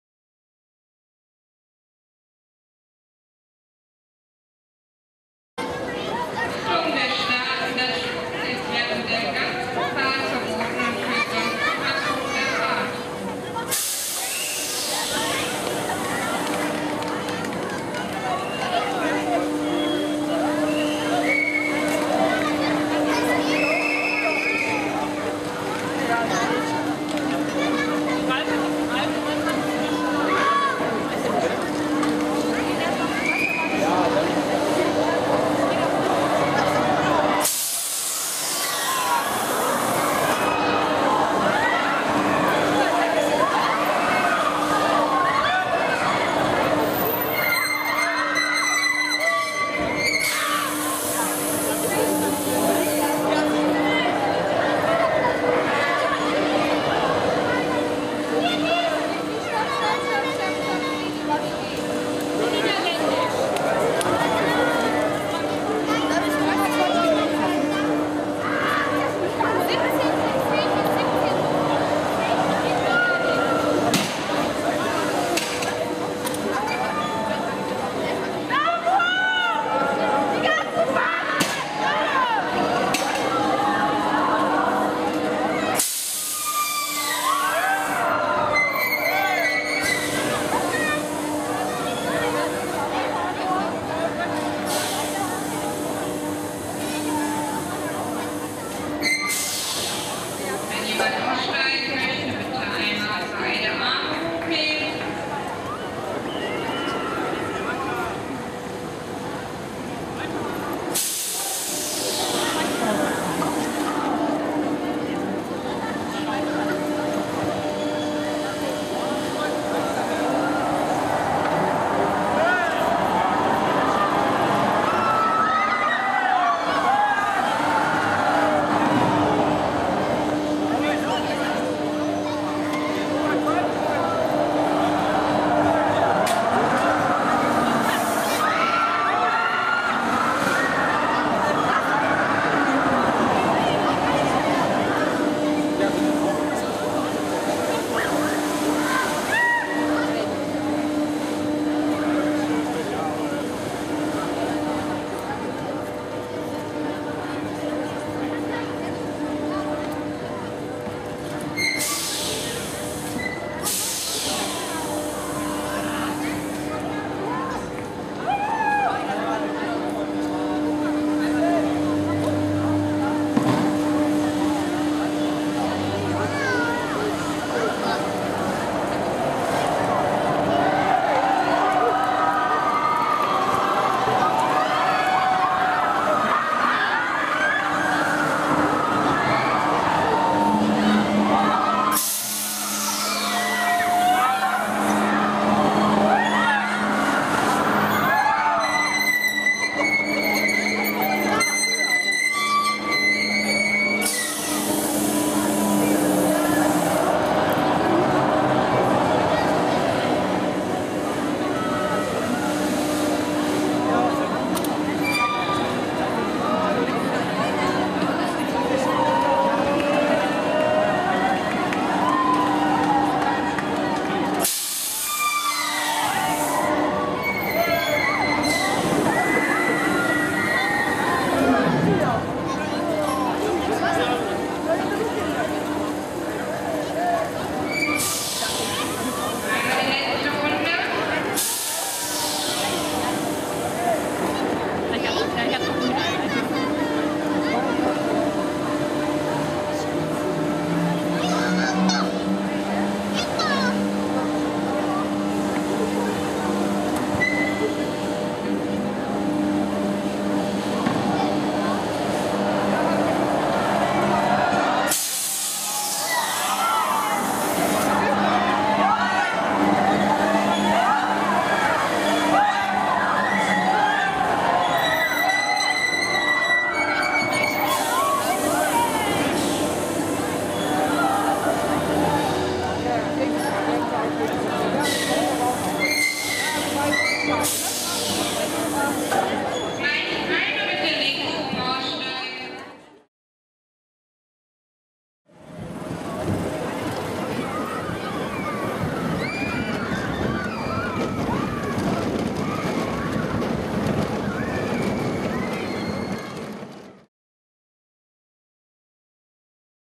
Movie Park: NYC Transformer [C4U - Offride]
Dies ist ein offride Video von dem Topspin "NYC Transformer" aus dem Movie Park Germany in Bottrop Kirchellen.